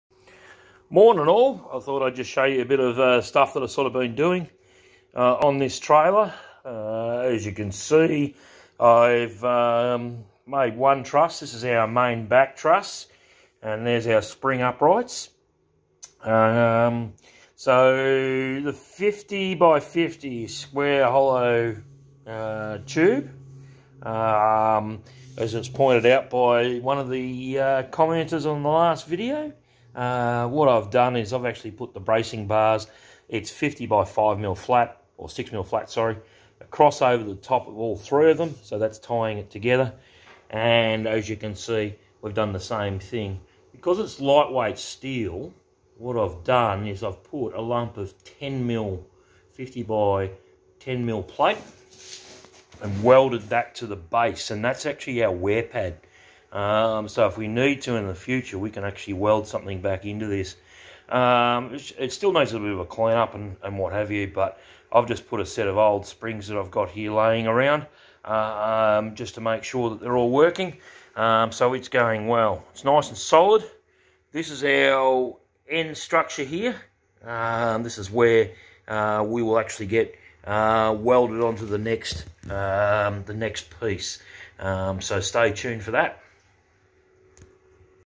Building the chassis rail
Using 50 x 50 x 2 mm SHS I've welded it into a laminated beam, added a end section to it and welded on spring hangers.
A wear plate for springs is also added